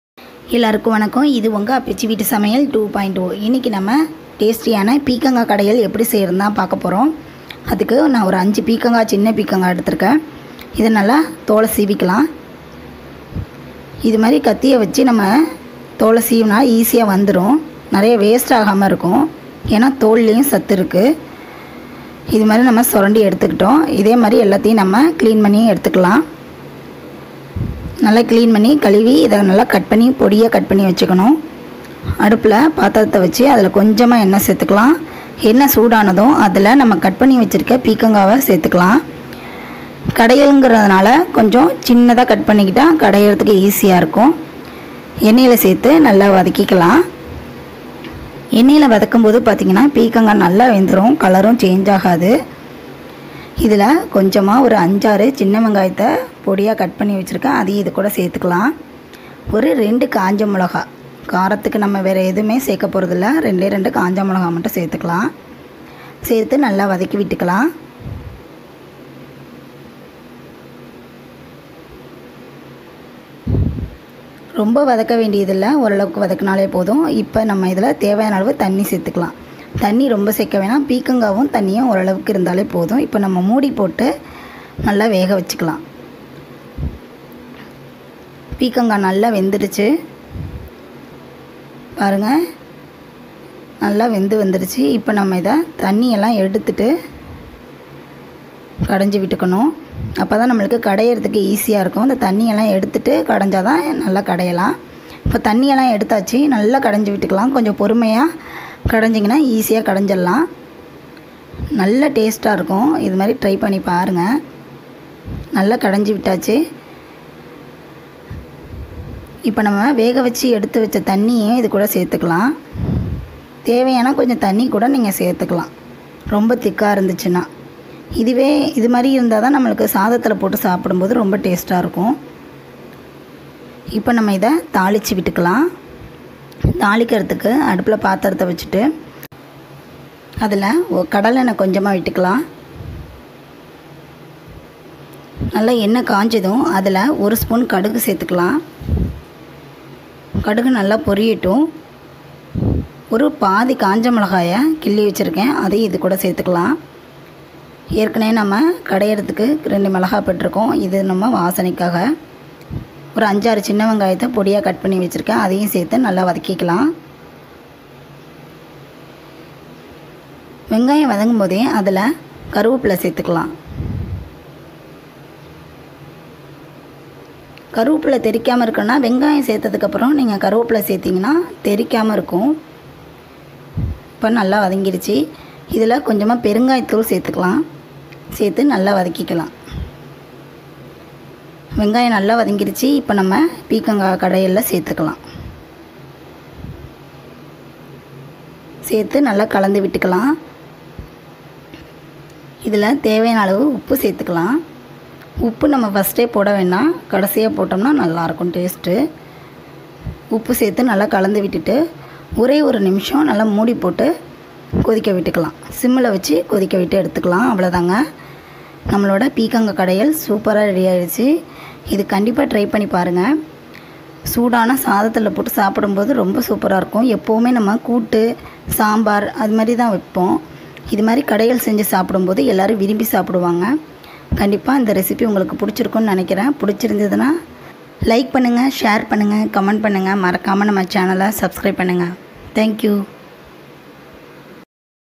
பீர்க்கங்காய் இருக்கா அப்போ இதை செஞ்சி பாருங்க | ridgegourd recipe | ridgegourd recipe in tamil
ingrediants:
ridgegourd - 5
rechilli - 2 or 3 required
mustard seeds - 1 spn
asafeodita- 2 pinch
curry leaves
salt as required
tasty kadaiyal for hot rice with potato fry
try it and taste it friends